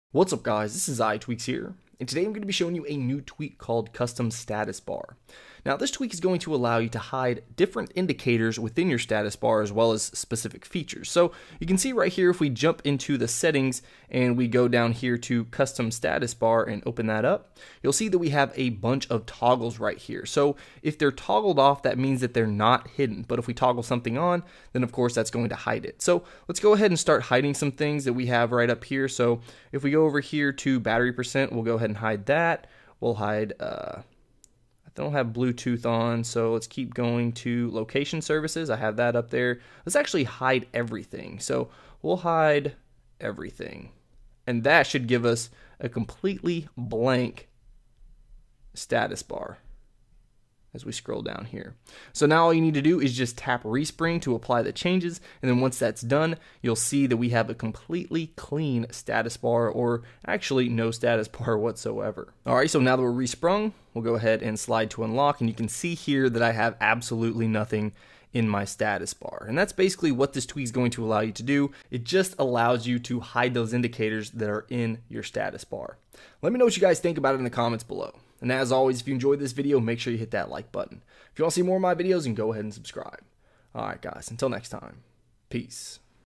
CustomStatusBar hides indicators in your Status Bar - iPhone Hacks
If you're looking to hide specific indicators or the entire status bar altogether, this is the tweak you want to check out.